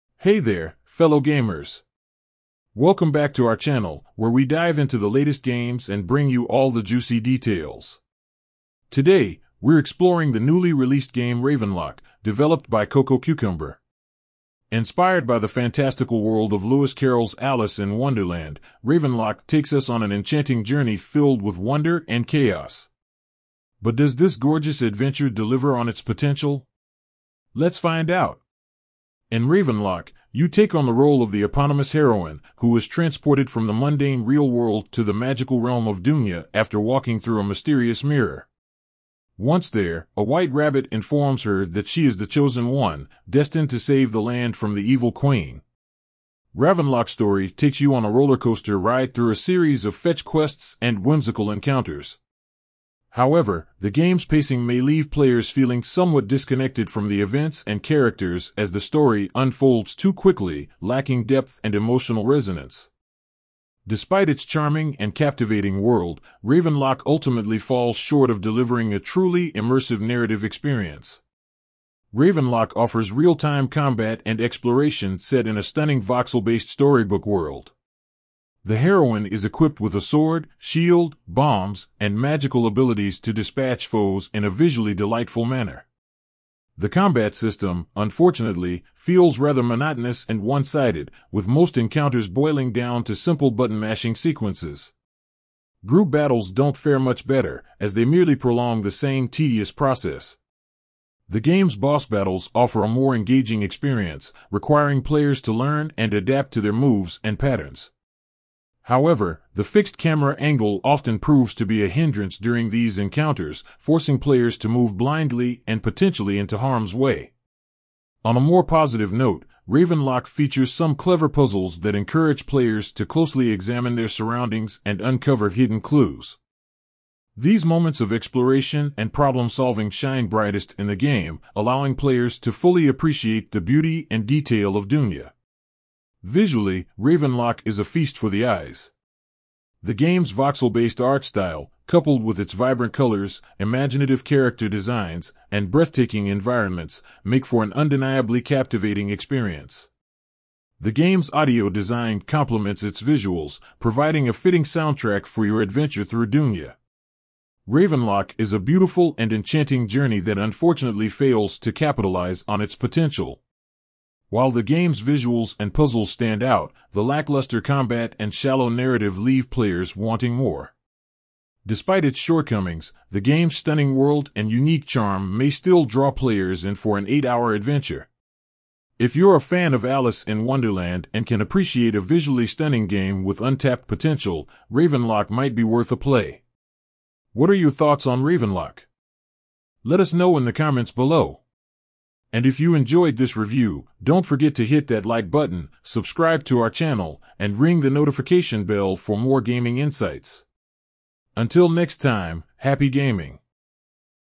Ravenlok - Game Review
Hey there, fellow gamers! Today, we're exploring the newly released game "Ravenlok," developed by Cococucumber. Inspired by the fantastical world of Lewis Carroll's Alice in Wonderland, Ravenlok takes us on an enchanting journey filled with wonder and chaos. But does this gorgeous adventure deliver on its potential? Let's find out!

In Ravenlok, you take on the role of the eponymous heroine, who is transported from the mundane real world to the magical realm of Dunia after walking through a mysterious mirror. Once there, a white rabbit informs her that she is the chosen one, destined to save the land from the evil Queen.

Ravenlok's story takes you on a rollercoaster ride through a series of fetch quests and whimsical encounters. However, the game's pacing may leave players feeling somewhat disconnected from the events and characters as the story unfolds too quickly, lacking depth and emotional resonance. Despite its charming and captivating world, Ravenlok ultimately falls short of delivering a truly immersive narrative experience.

Ravenlok offers real-time combat and exploration set in a stunning voxel-based storybook world. The heroine is equipped with a sword, shield, bombs, and magical abilities to dispatch foes in a visually delightful manner. The combat system, unfortunately, feels rather monotonous and one-sided, with most encounters boiling down to simple button-mashing sequences. Group battles don't fare much better, as they merely prolong the same tedious process.

The game's boss battles offer a more engaging experience, requiring players to learn and adapt to their moves and patterns. However, the fixed camera angle often proves to be a hindrance during these encounters, forcing players to move blindly and potentially into harm's way.

On a more positive note, Ravenlok features some clever puzzles that encourage players to closely examine their surroundings and uncover hidden clues. These moments of exploration and problem-solving shine brightest in the game, allowing players to fully appreciate the beauty and detail of Dunia.

Visually, Ravenlok is a feast for the eyes. The game's voxel-based art style, coupled with its vibrant colors, imaginative character designs, and breathtaking environments, make for an undeniably captivating experience. The game's audio design complements its visuals, providing a fitting soundtrack for your adventure through Dunia.

Ravenlok is a beautiful and enchanting journey that unfortunately fails to capitalize on its potential. While the game's visuals and puzzles stand out, the lackluster combat and shallow narrative leave players wanting more. Despite its shortcomings, the game's stunning world and unique charm may still draw players in for an 8-hour adventure. If you're a fan of Alice in Wonderland and can appreciate a visually stunning game with untapped potential, Ravenlok might be worth a play.

What are your thoughts on Ravenlok? Until next time, happy gaming!